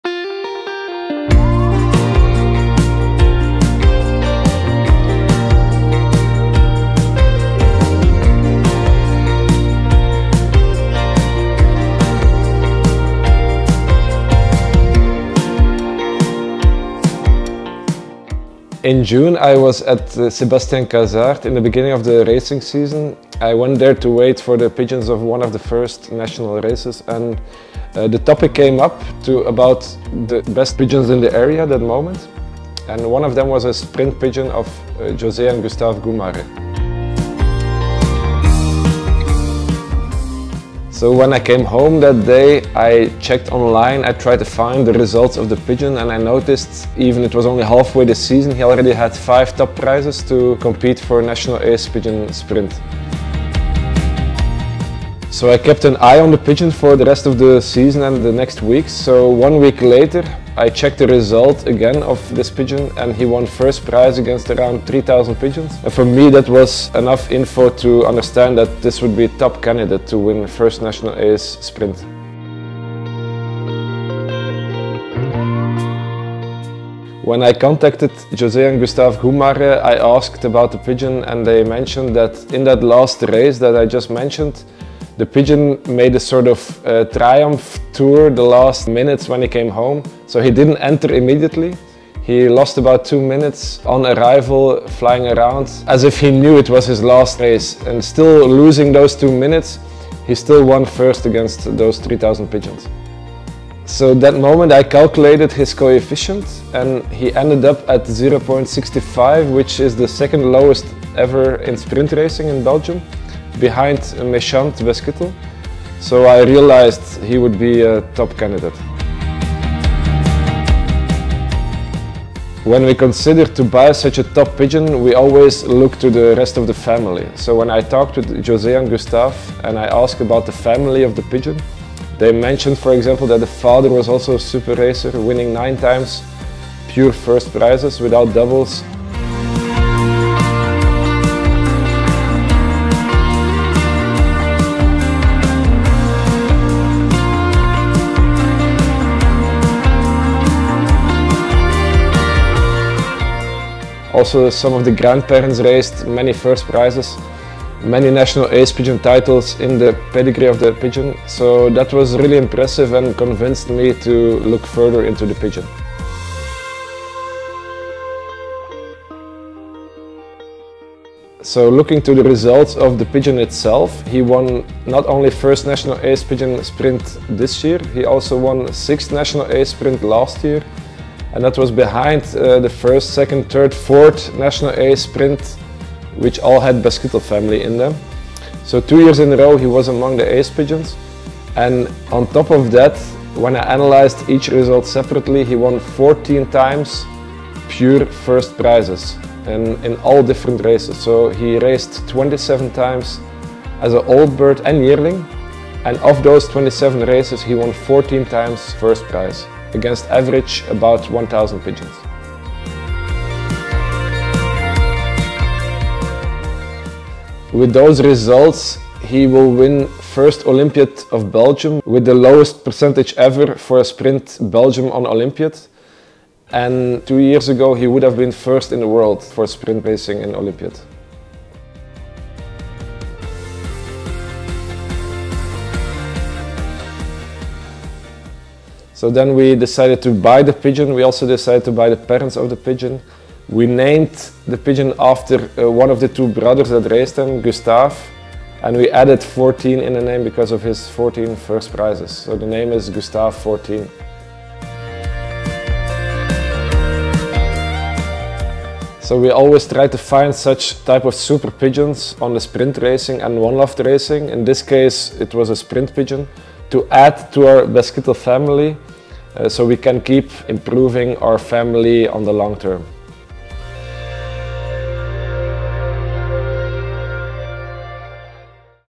PIPA Breeding introduces 'Gustave XIV'
“Gustave XIV” wins 14x pure 1st prizes on sprint racing in Belgium against average +1.000 pigeons, a unique palmares that made him 1. Olympiad Sprint Cat. A Belgium for the upcoming Olympiad 🏆. He even became 2nd best Olympic pigeon worldwide on the sprint. It’s very rare to have a Belgian pigeon in the top 3 of the world ranking for sprint.
PIPA Breeding bought this pigeon from José & Gustave Goemaere. Thomas introduces the pigeon to you.